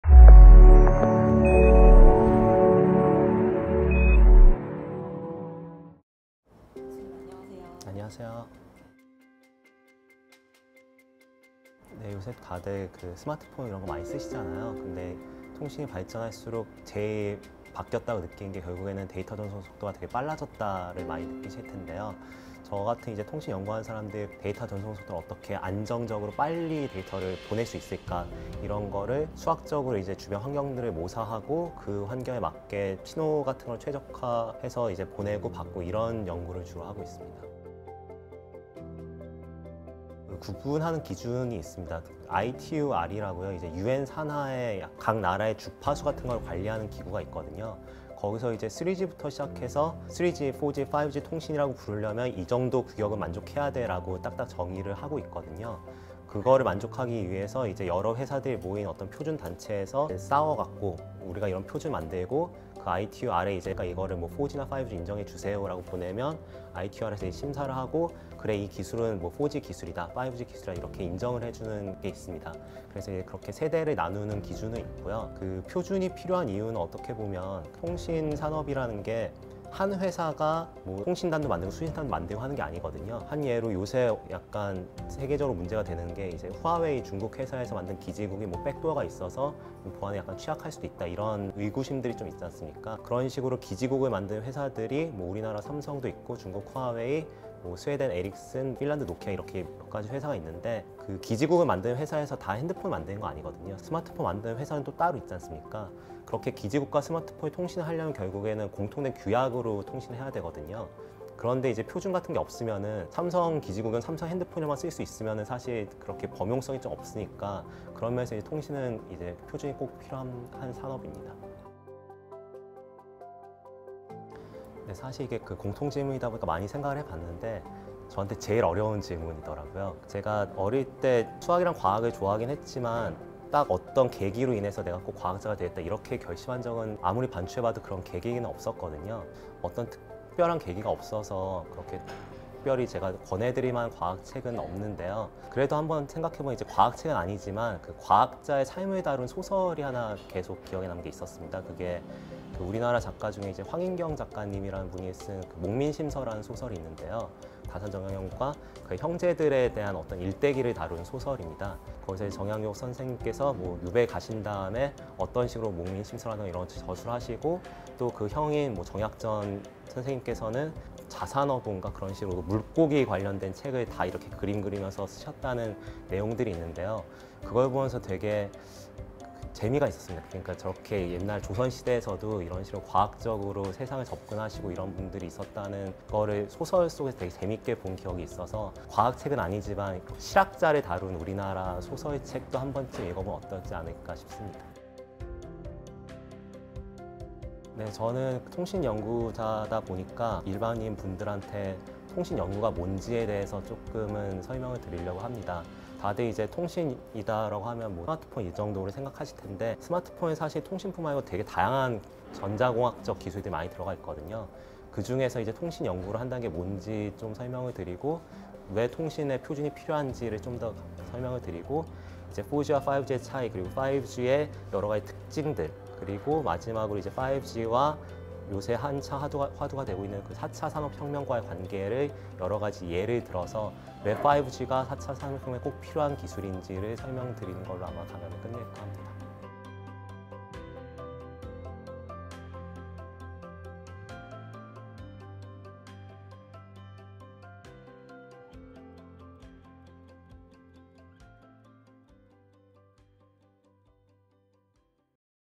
[석학인터뷰] 최준일_ 4G, 5G, 6G??? 그래서 그게 뭔데?! | 2020 봄 카오스강연 '첨단기술의 과학'
[석학인터뷰] 4G, 5G, 6G??? 그래서 그게 뭔데?! | 최준일 카이스트 전기 및 전자공학부 교수 |  2020 봄 카오스강연 '첨단기술의 과학'

최준일 교수님과 함께하는 봄 카오스강연 '첨단기술의 과학'이 4월 8일 오늘 저녁 7시!! 생중계됩니다!

장소제공: 북파크 라운지